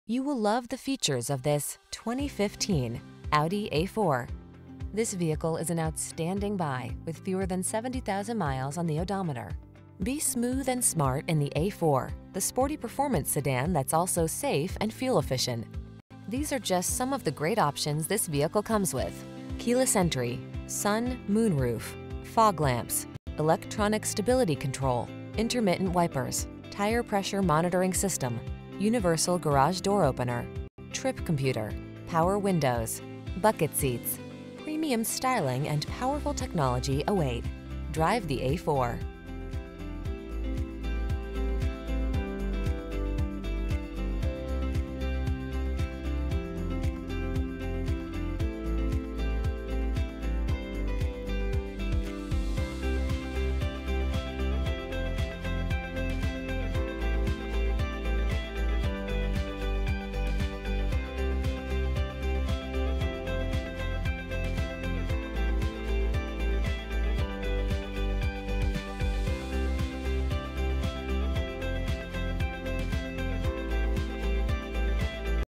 2015 Audi A4 at Oxmoor Mazda | Louisville & Lexington, KY MU7000
Brilliant Black Used 2015 Audi A4 available at Oxmoor Mazda in Louisville, Kentucky. Servicing the Lexington, Elizabethtown, KY New Albany, IN Jeffersonville, IN area.
2015 Audi A4 2.0T Premium - Stock#: MU7000 - VIN#: WAUAFAFL4FN020836

Oxmoor Mazda
7913 Shelbyville Rd

2015 Audi A4 2.0T Premium Vehicle is at OXMOOR MAZDA.